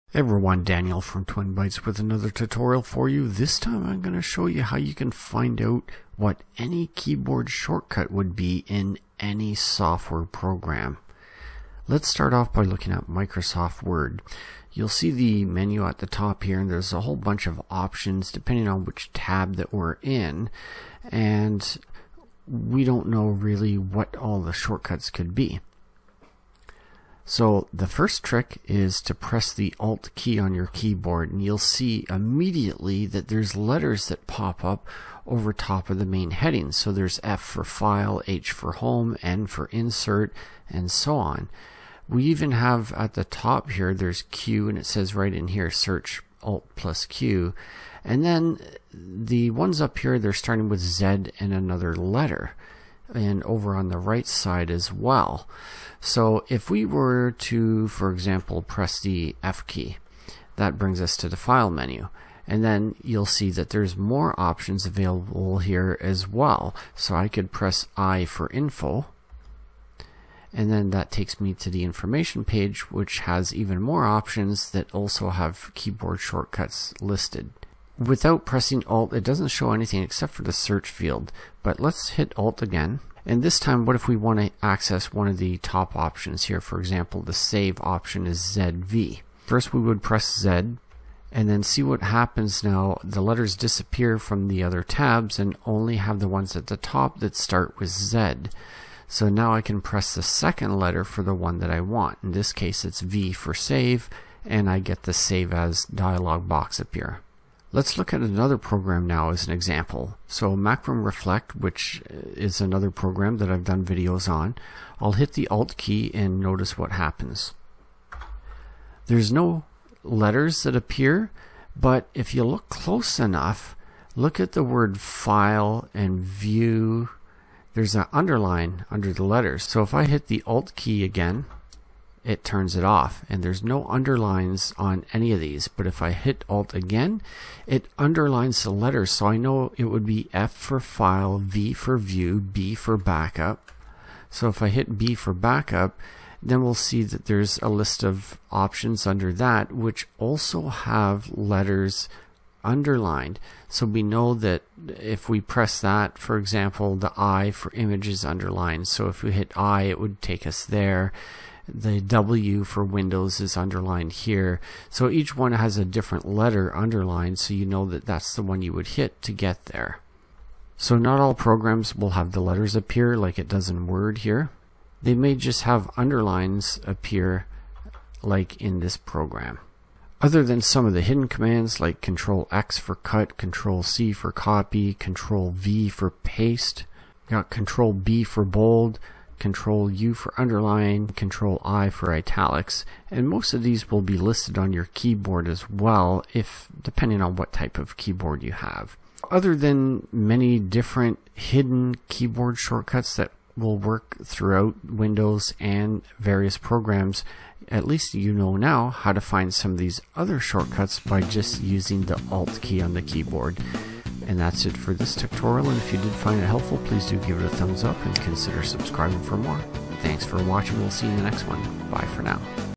How to find keyboard shortcuts in any program
If you want to know how to find any keyboard shortcut easily in any program, watch this video to find out how mind blowing easy it is.
There's more than one way so watch the entire video which is also a great way of showing your support for my efforts.

CHAPTERS
0:00 Intro
0:15 Programs like Word
1:47 Programs like Macrium
2:50 common shortcuts